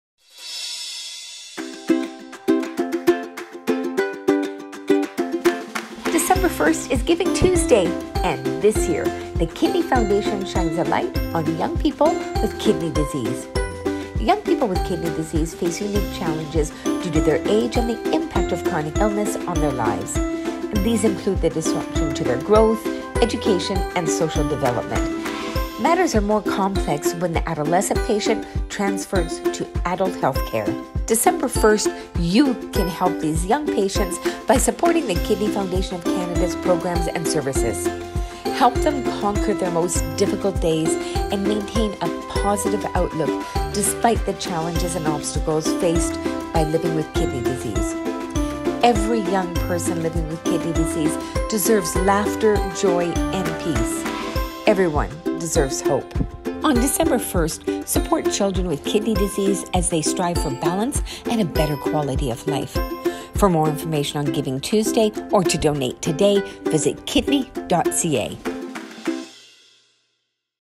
Dec 1st is Giving Tuesday. Learn how YOU can help support children living with kidney disease
Young people with kidney disease face unique challenges due to their age and the impact of chronic illness on their lives. These include disruption to their growth, education and social development. Matters are more complex when adolescent patients transfer to adult healthcare.

This December 1st YOU can help these young patients by supporting The Kidney Foundation of Canada’s programs and services. Help them conquer their most difficult days and maintain a positive outlook despite the challenges and obstacles faced daily with kidney disease.

Every young person living with kidney disease deserves laughter, joy and peace.  Everyone deserves hope.

On December 1st, please support children with kidney disease as they strive for balance and a better quality of life.

For more info on how to become an organ donor,